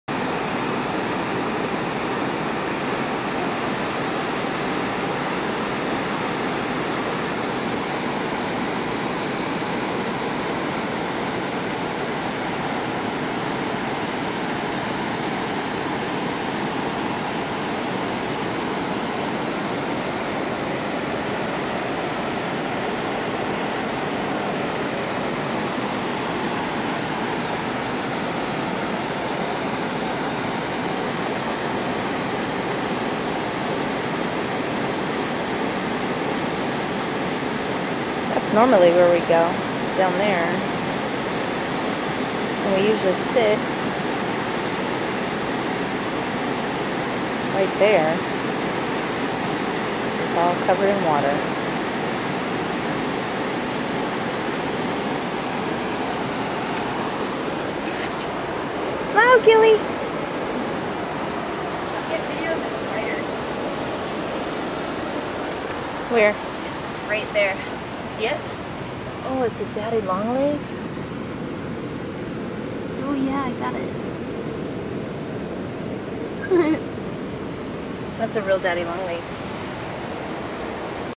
The Perdenales River Flowing Sunday , May 17th.
The Perdenales River at Johnson City , Texas. really doing it's thing.